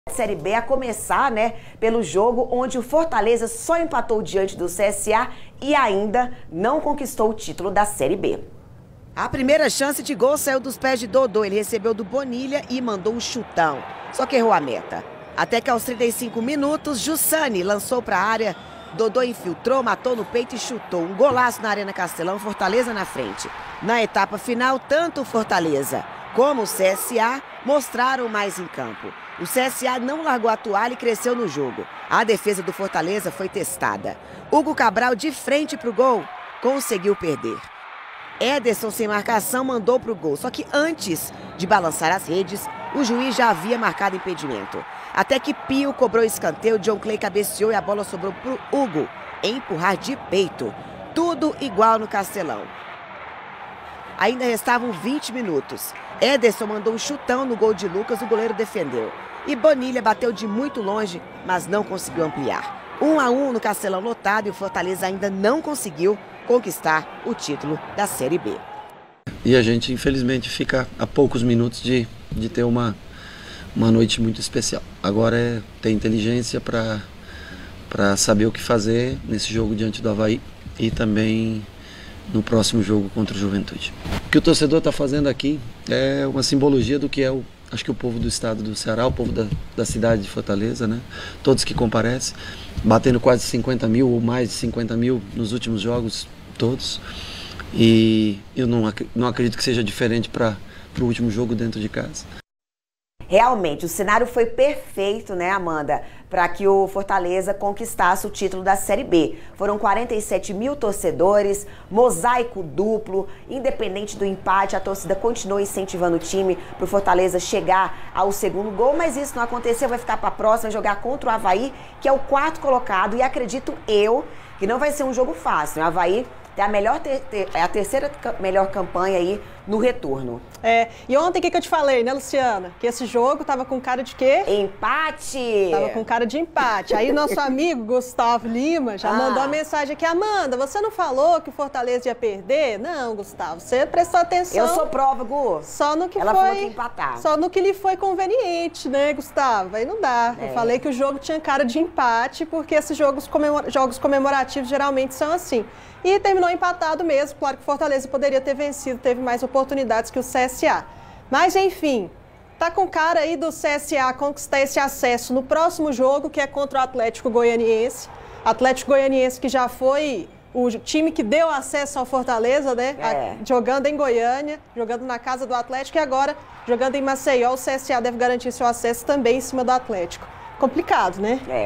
Fortaleza adia título da série B após ficar no empate diante do CSA
O Leão saiu na frente, mas viu o CSA crescer no jogo e conquistar o empate.

Brasil Esportes de segunda a sexta de 12:00 às 12:30 e aos domingos de 22:00 às 23:00. Para todo território nacional pela TV Aparecida.